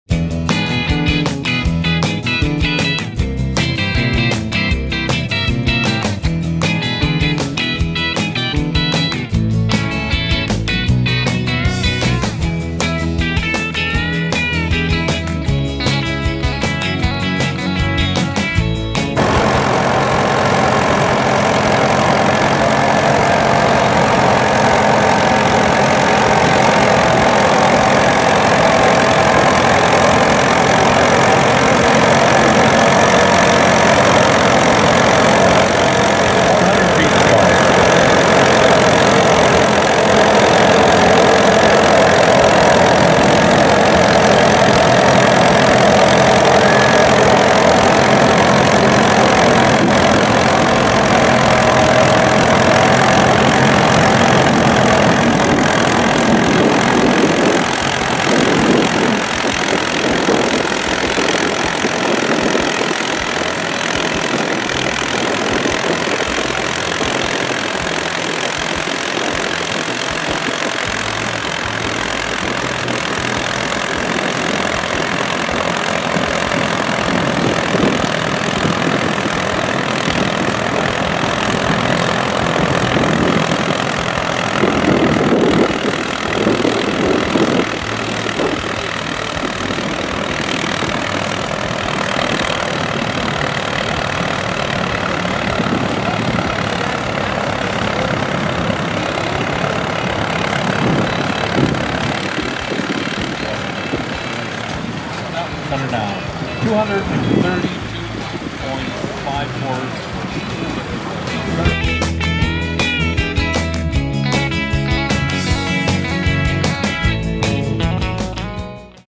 Ol' Pembina (Sangudo) June 2024
Tom Speedie  Case DC4  Class 2
1st pull 232' 5"  Tractor Weight 5960 lbs